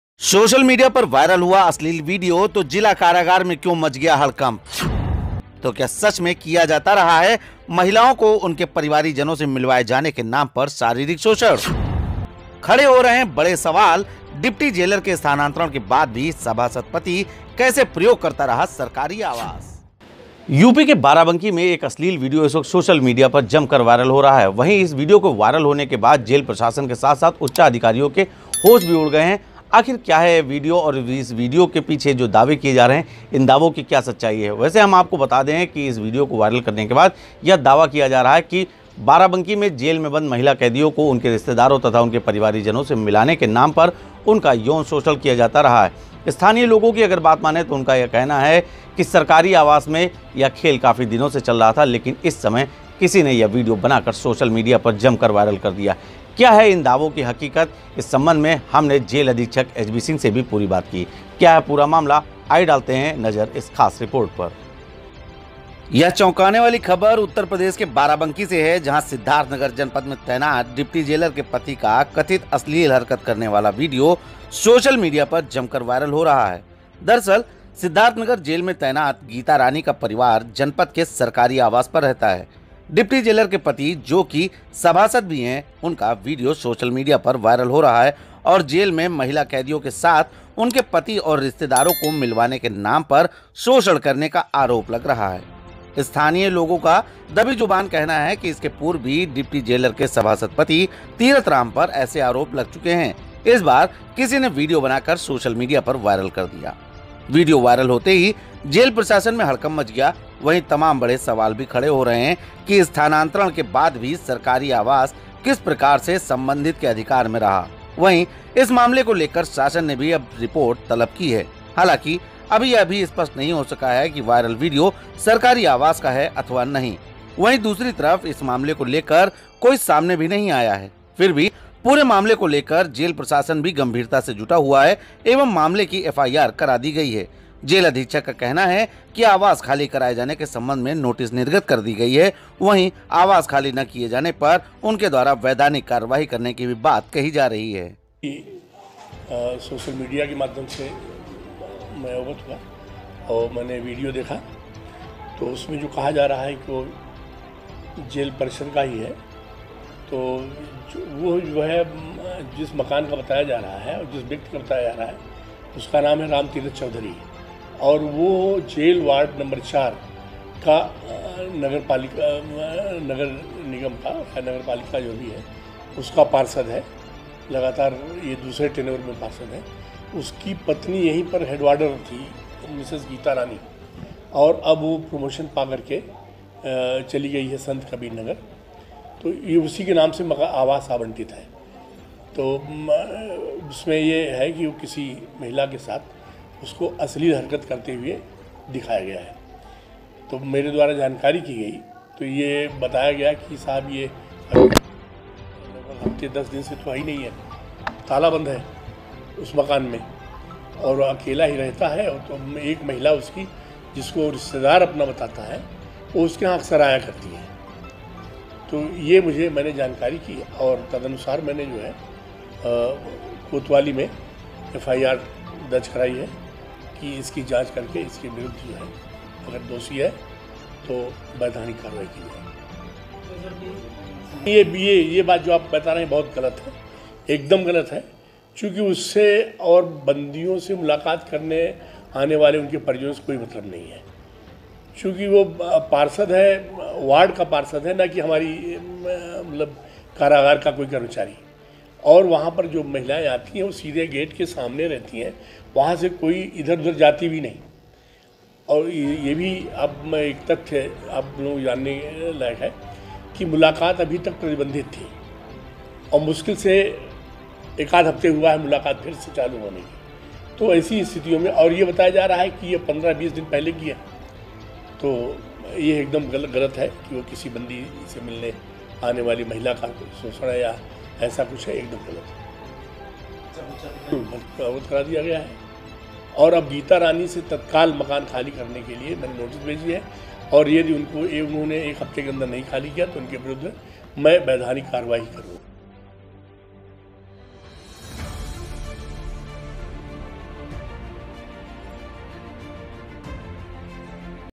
सरकारी आवास में डिप्टी जेलर का सभासद पति करता रहा महिलाओं का शोषण? वायरल वीडियो से सामने आयी करतूत
गैर जनपद तैनात महिला डिप्टी जेलर का सभासद पति जेल में मिलाई के नाम पर करता रहा महिलाओं का शोषण? वायरल वीडियो के साथ किया जा रहा दावा! जानिए मामले की पूरी हकीकत, क्या बोले जेल अधीक्षक?